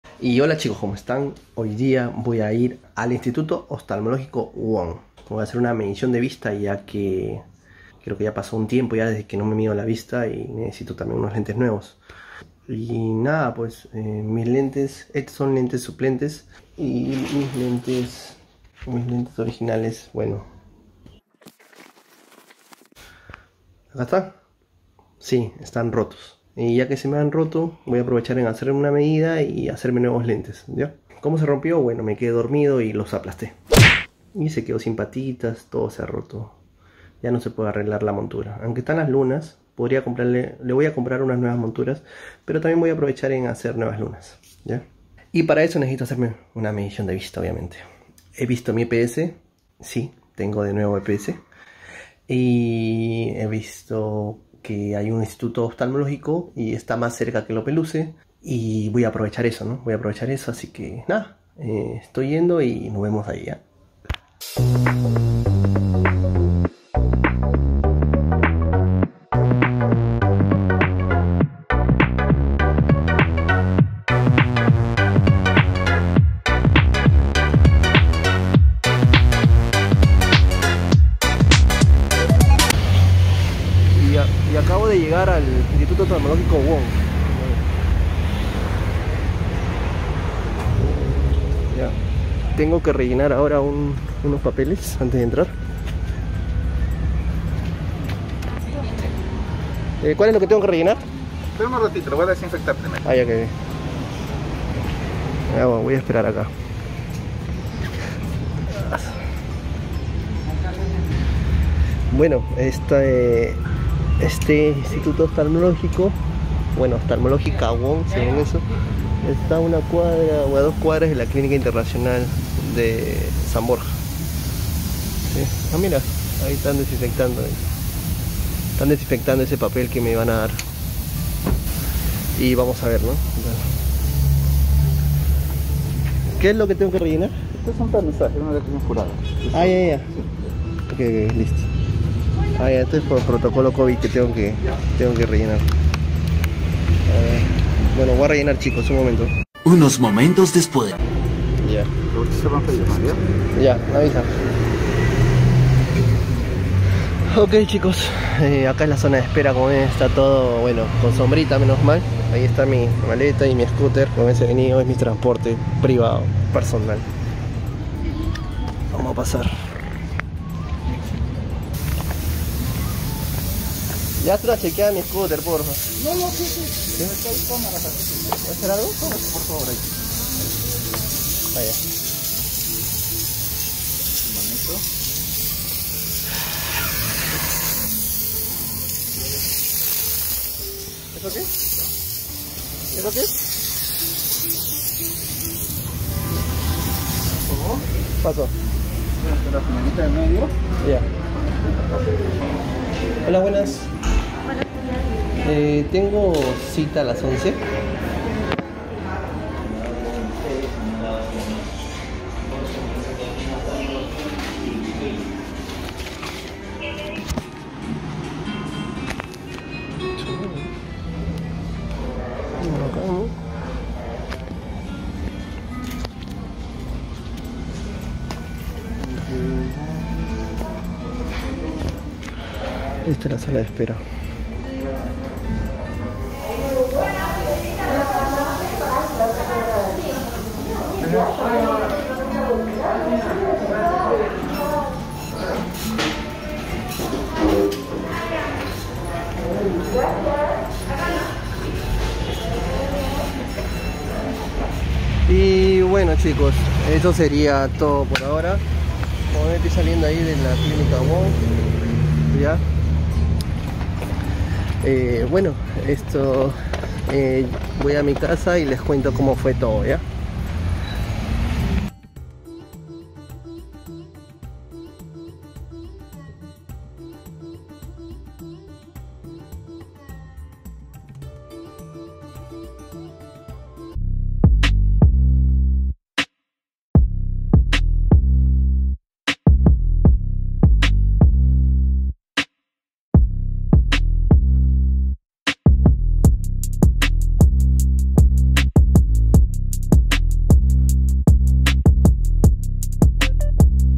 PLACES #9 || ME VOY A MEDIR LA VISTA || WONG OFTALMÓLOGOS
Hola chicos, en esta oportunidad les muestro el día que fuí a medirme la vista al Instituto Oftalmológico Wong. Espero les guste y me dejen su like.